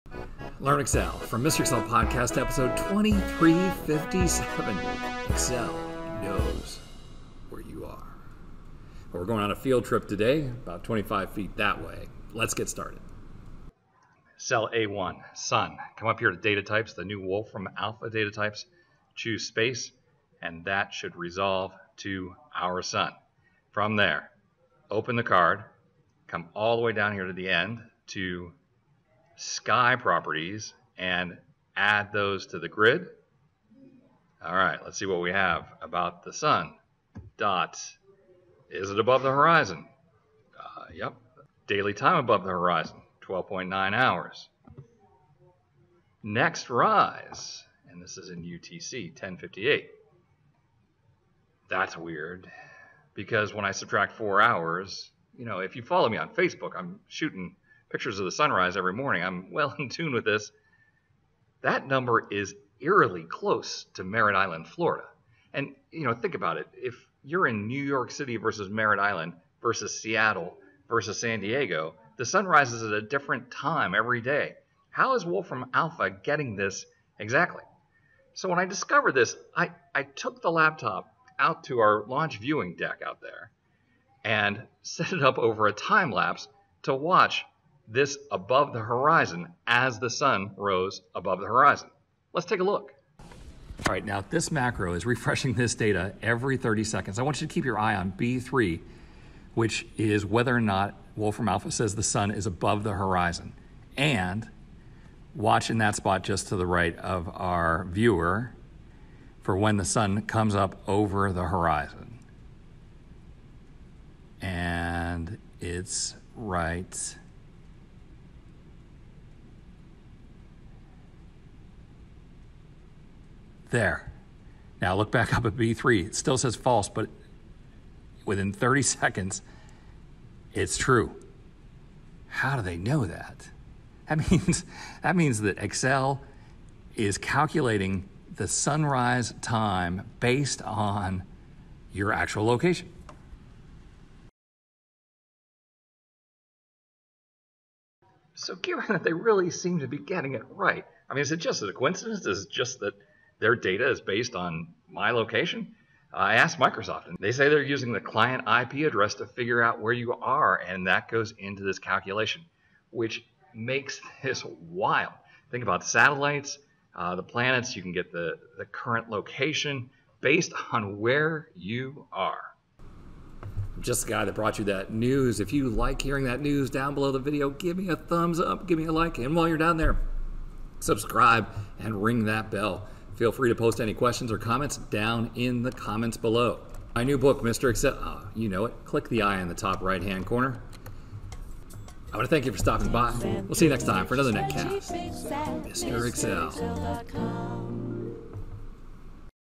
Excel Location Awareness: Excel Knows Where You Are - Episode 2357
Microsoft Excel Tutorial: Does Excel Know your geographic location?

Experimenting with the Wolfram Alpha data types, I discovered that Excel can predict when the Sun will rise at your location with amazing accuracy. Since the sunrise time is based on Latitude and Longitude of the viewer, this means that Excel must know where you are located, based on the I.P. address of your computer.

In this episode of the MrExcel podcast (episode 2357), Bill Jelen takes the viewers on a field trip to explore a fascinating feature in Excel. He begins by mentioning that Excel "knows where you are" and demonstrates this using the new Wolfram Alpha data types.

Bill instructs viewers to type "Sun" in cell A1 and then uses the Wolfram Alpha data types to select "Space," which resolves to information about the Sun. He explores various details about the Sun, such as its daily time above the horizon and the next sunrise time (in UTC).

What piques Bill's interest is the accuracy of the sunrise time provided by Wolfram Alpha. He notes that it is eerily close to the actual sunrise time in Merritt Island, Florida, where he resides. This leads him to investigate how Wolfram Alpha obtains this precise data.

To test the accuracy, Bill sets up a time-lapse video of the sunrise on Excel's data refresh rate. He observes that within 30 seconds of the Sun rising, Excel updates the "Above the Horizon" field to TRUE. This revelation prompts him to inquire further into how Excel determines the user's location for calculating sunrise times.

Bill explains that Microsoft uses the client's IP address to determine their location, which is then used to calculate sunrise times. He finds this capability intriguing, particularly for tracking satellites, the International Space Station (ISS), and planets' locations.

He also mentions his new book, "MrExcel," and invites questions and comments from the audience.

This episode showcases Excel's ability to provide location-based data and highlights the precision with which it calculates sunrise times based on the user's location.

This video answers these common search terms:
how does excel know your location
privacy in excel
privacy from microsoft

(0:00) Problem Statement: Excel Privacy
(0:10) Wolfram Alpha Data Type for Sun & Show Sunrise
(1:00) How does Excel know when the sun rises?
(1:24) Time Lapse of Laptop & Sunrise
(2:20) Excel has to know your actual location
(2:43) Excel uses location address of your IP
(2:55) Clicking Like really helps with the algorithm